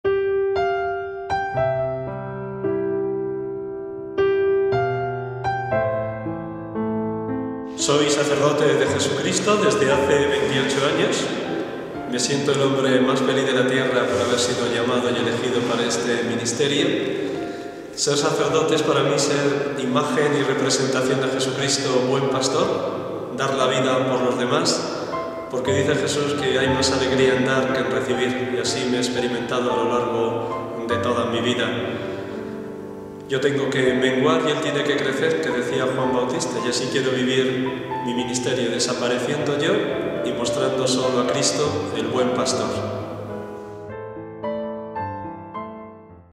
MIGUEL ANGEL ARRIBAS
Ofrecemos el testimonio de Miguel Angel Arribas, sacerdote de nuestra diócesis y ordenado en 1985